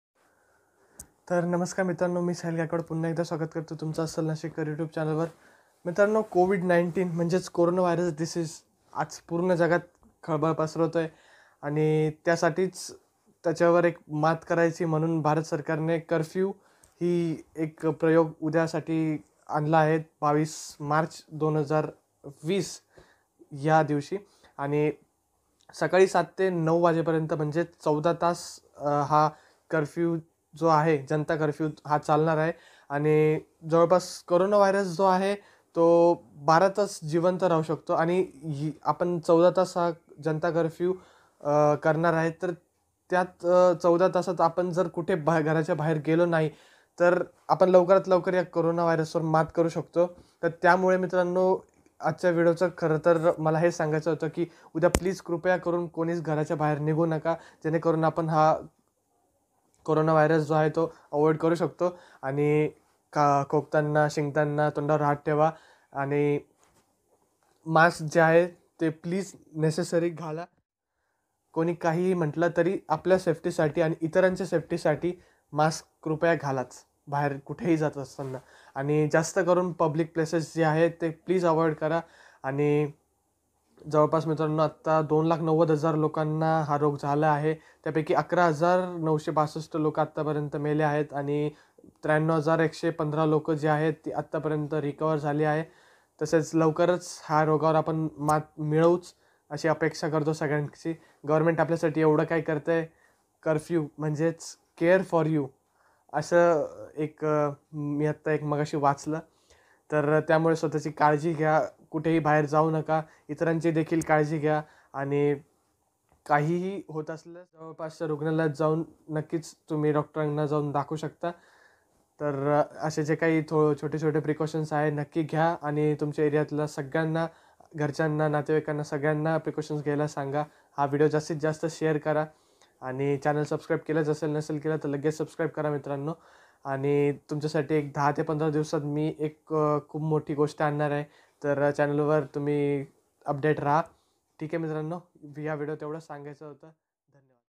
Janta Curfew 2020 | Corona Virus Govt. project Must watch video now
Mitrano  khup divs zhle hodeo ala navhta tyabddal sorry mala hya video madhun evdhch sandesh dyaychy ki pls udya 22 march 2020 gharatun baher nighu naka

Ha sandesh jastit jasta lokana dya video share kara like kara ani comment kara ani tasech mala Instagram vr follow kra ani msg kara me nakkich tumhla reply krel kahi ideas astil mazhasobat share kara me nakkich tyavr video banvel

Dhanyawad  mitrano

Asch support nehmich asu dya

Tumchach,

Sahil Gaikwad (S.M.G.)